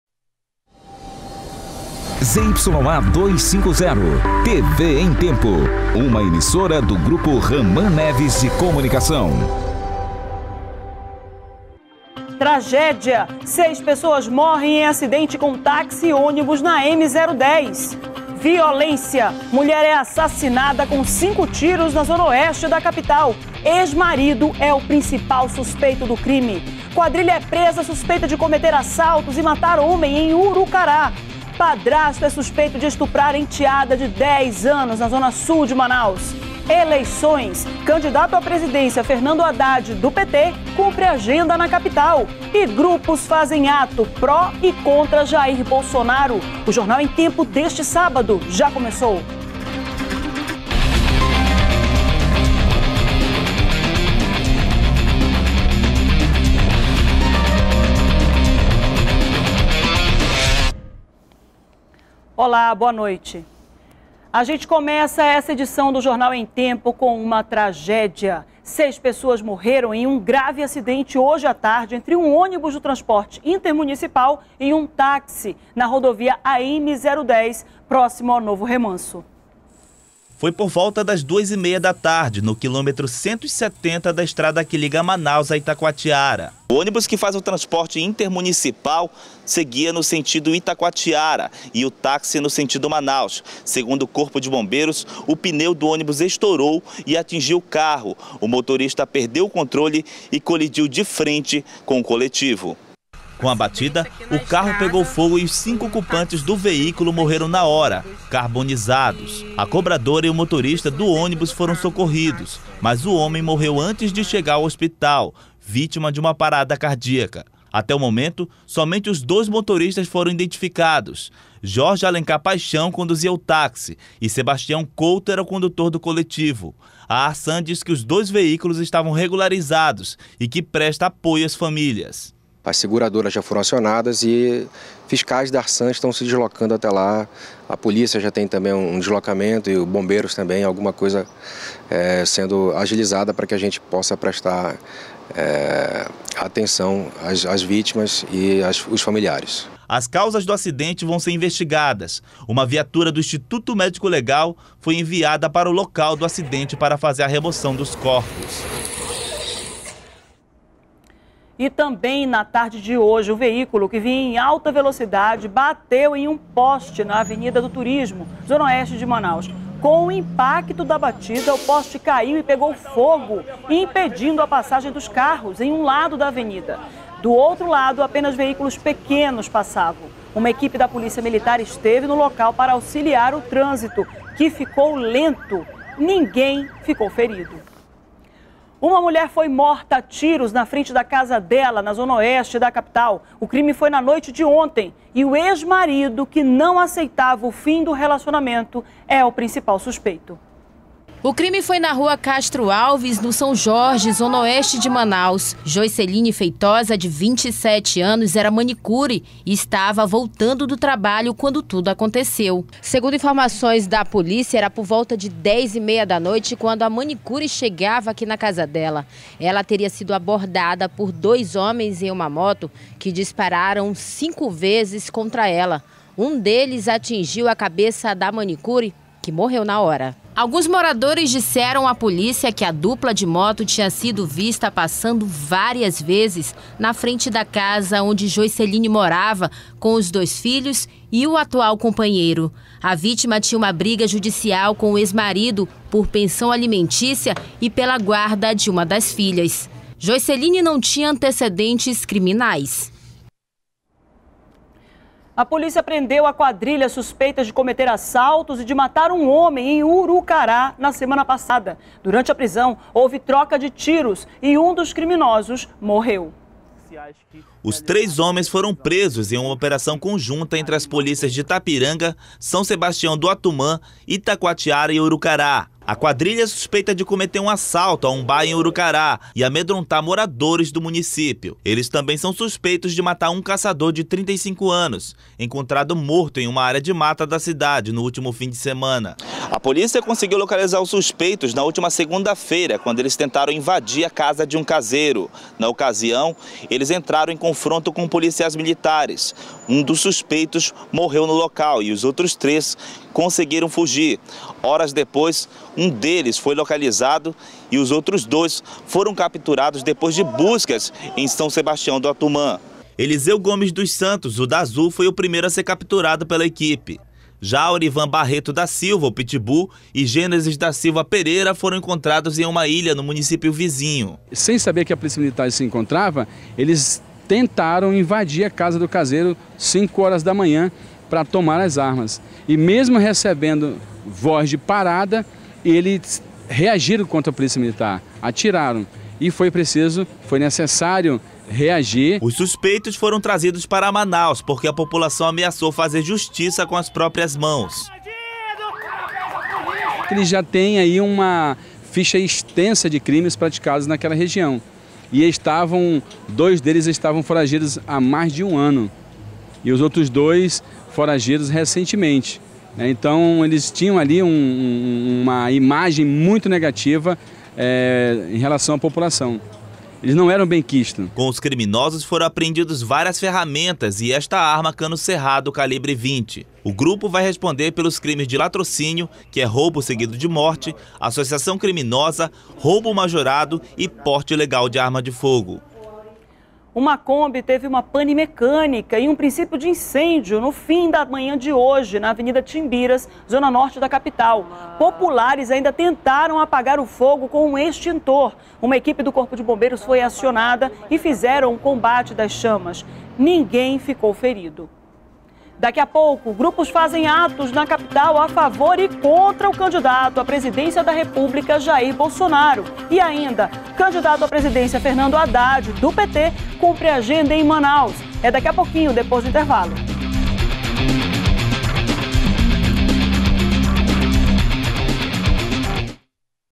Tragédia! Seis pessoas morrem em acidente com taxi e ônibus na AM-010
01º BLOCO JORNAL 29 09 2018 - Tragédia! Seis pessoas morrem em acidente com taxi e ônibus na AM-010